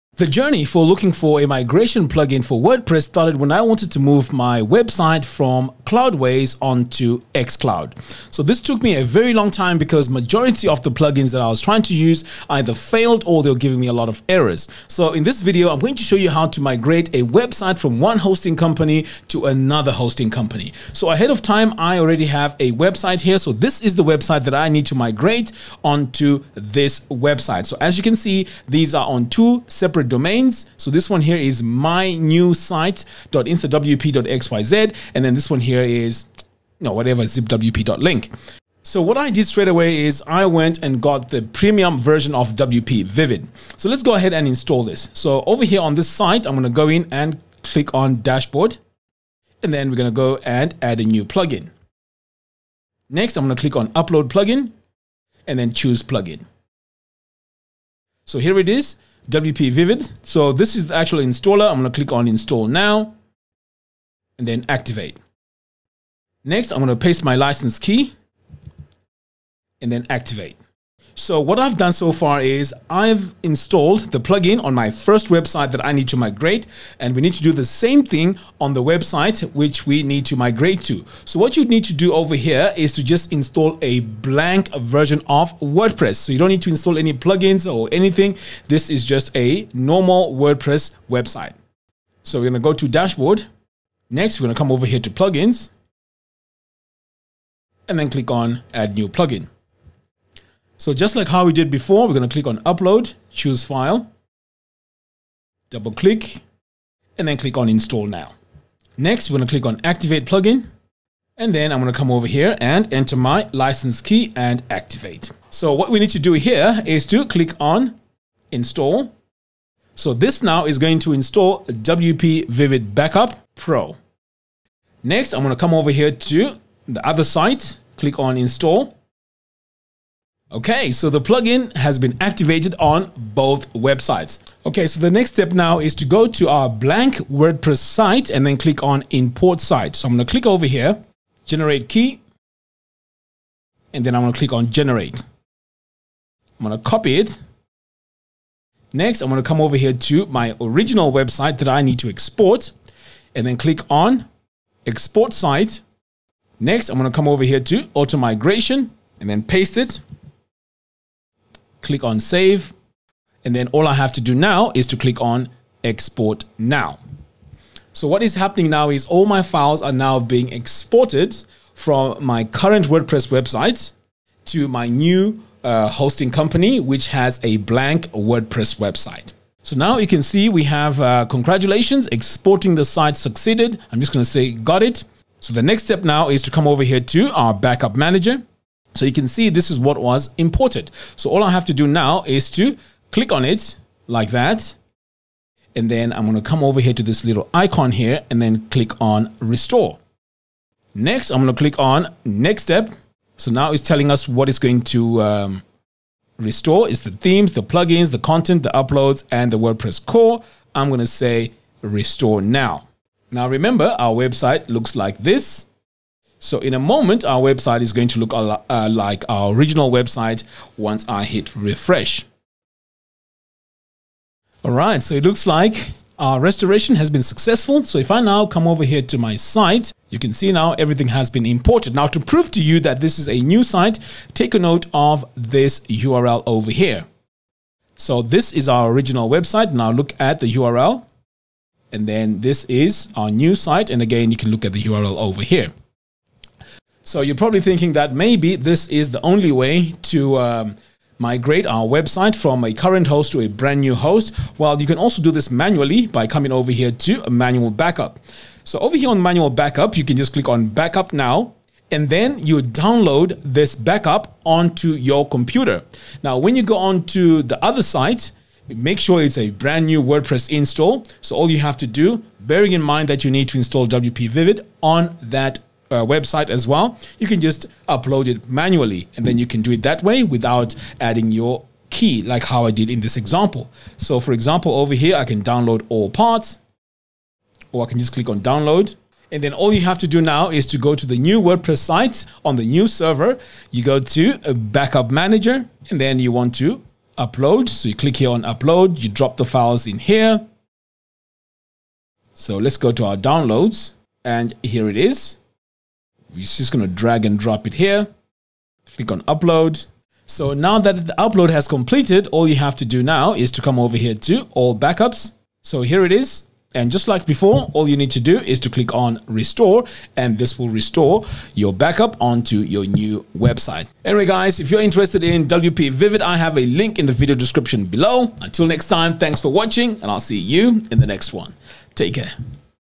How to migrate a wordpress site from one host to another - With WPVivid
In this video, I show you how to migrate a WordPress site from one host to another. For this I use WPVivid. I tried how to migrate a WordPress site from one host to another and it worked without any issues.

00:00 Introduction
00:47 How to install WPVivid
01:26 Install WPVivid on the destination website
02:20 Generate the Key on the destination website
02:38 Export website to destination website
03:12 Restore the website on the destination website
03:59 Test WordPress migration
04:16 How to manually migrate your WordPress website